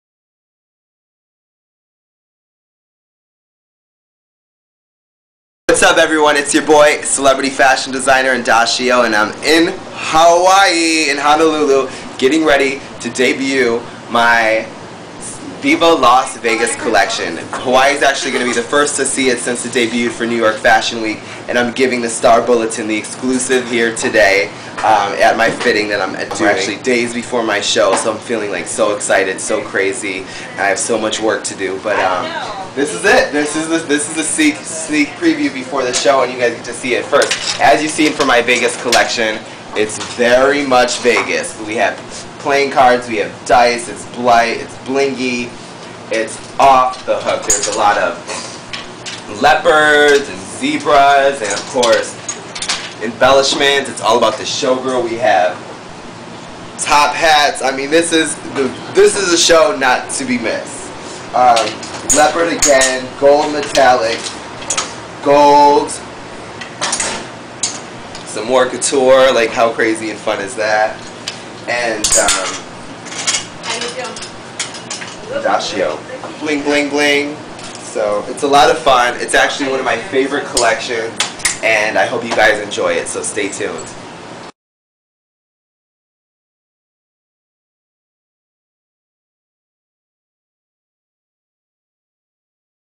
Indashio talks up his Honolulu fashion show
Designer from MTV's "8th & Ocean," "Made," and VH1's "Sew Good" prepares to stage his Jan. 12, 2008 "Viva Las Vegas" fashion show at Honolulu Design Center, with model Nalani Itomura. His Spring 2008 collection featuring gold lame, see-through dresses, animal prints and showgirl headwear debuted at New York Fashion Week in September 2007. Footage by Nadine Kam.